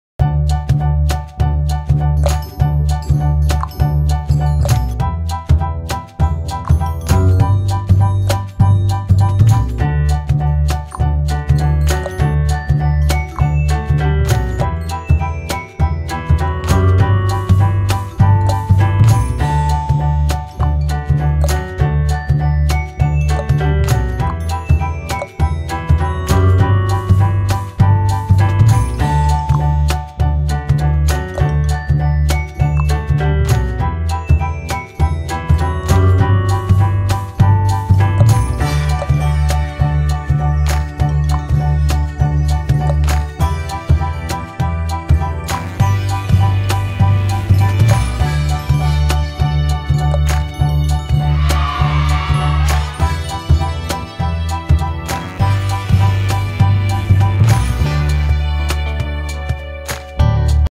Paint By Numbers | Rooster | Painting for Kids
Let's learn how to color a roster by numbers. Come and paint with me)